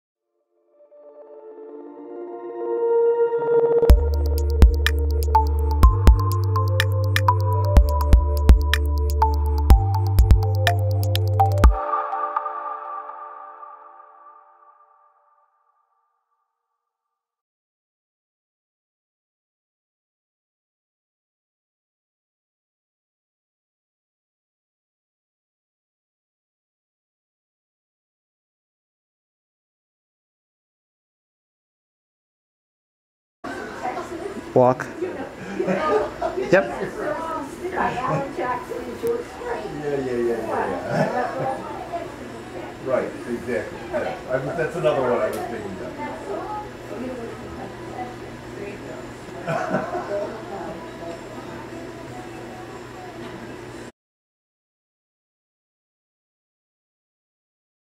Oralase / Smilelase , Fotona Laser
Patient with Multiple Sclerosis ( MS ) has improved neuromuscular control of her walking after therapeutic laser light treatment .